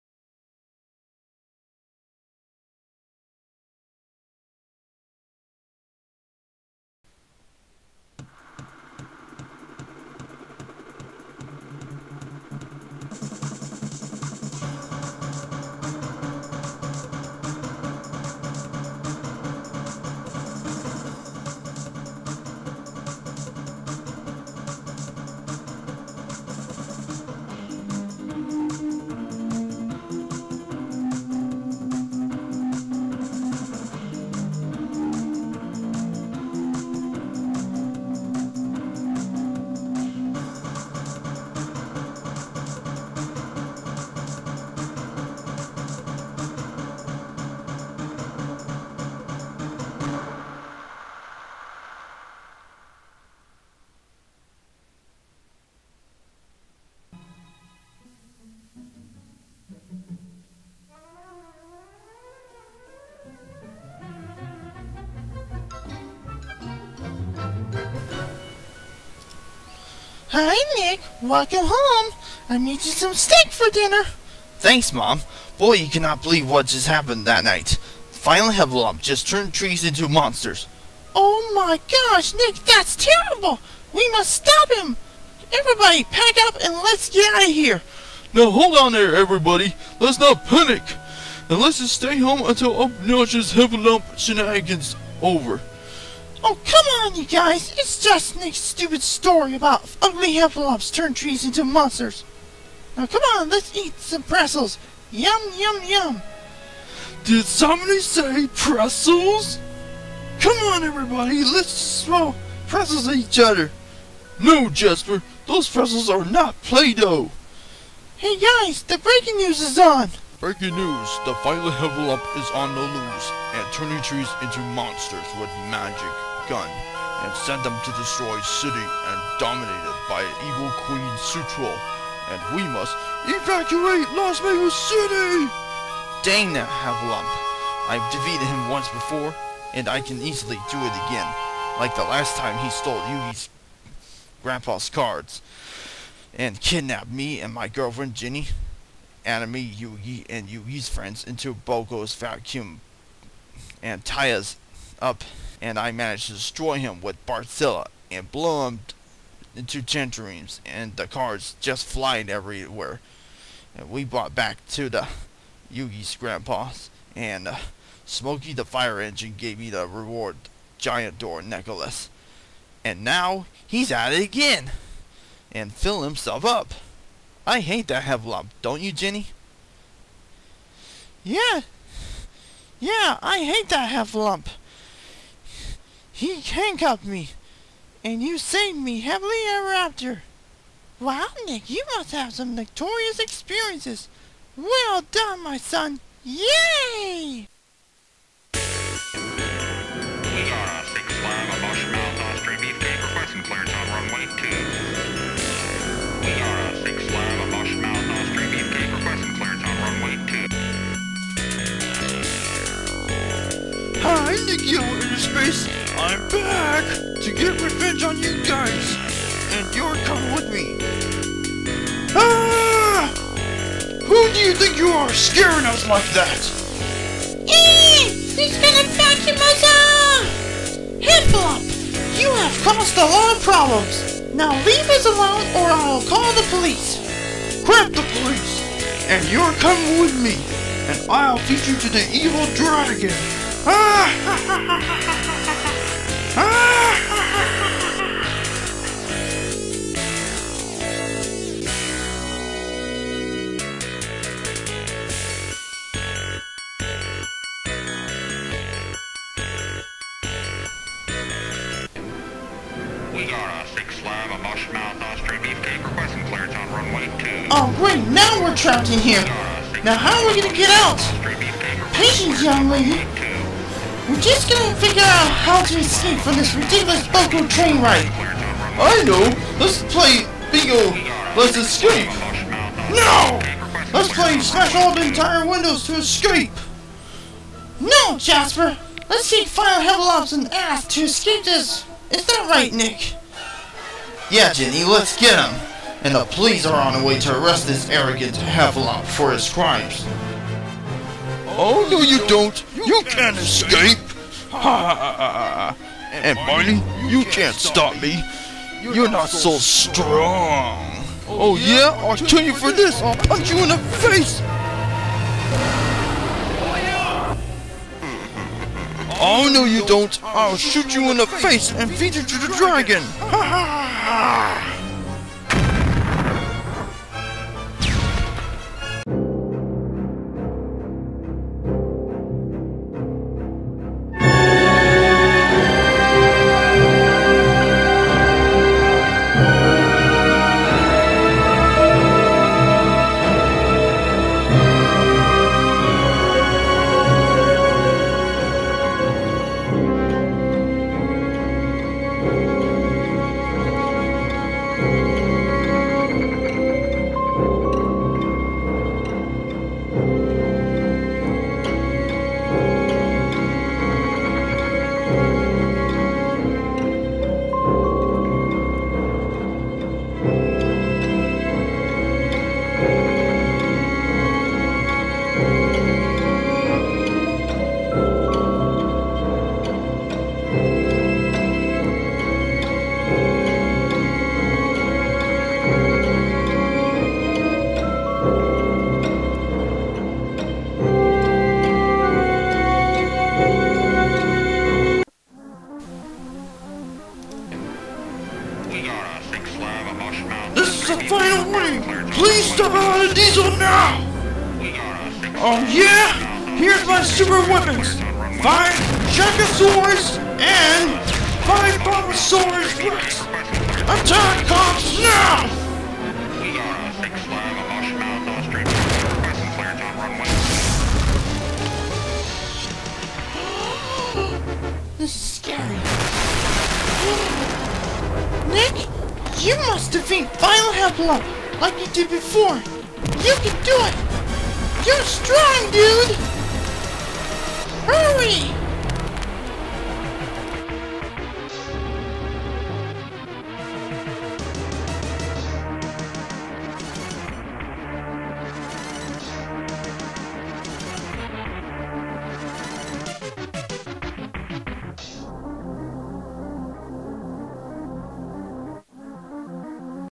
Adventure Man Season 1 Episode 1: Pilot Episode Part 1
Original Upload (March ‎05, 2016)

Plot - Violent Heffalump has turn trees into monsters to terrorize the city of Las Mavis California & Nick & his family stopped him first and then the Evil Queen Sutro showed up to turn into the dragon to destroy the city Nick and Mavis ordered the Robots and Monsters to fight back against other Monsters and save the city coming up next on part 2.